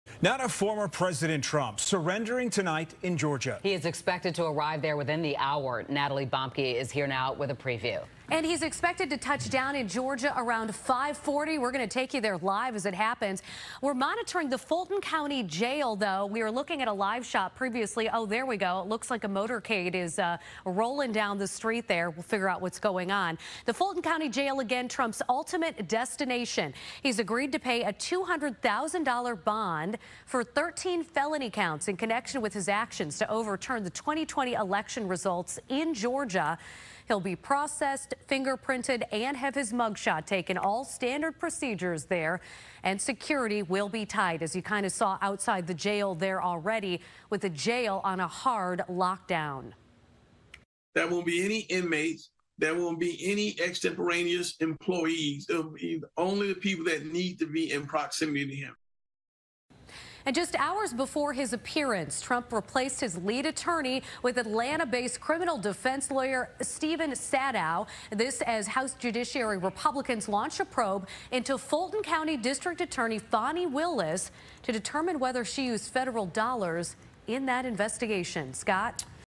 Trump to turn himself in Thursday night.
President Donald Trump is expected to surrender himself Thursday night in Georgia. Here is the latest update.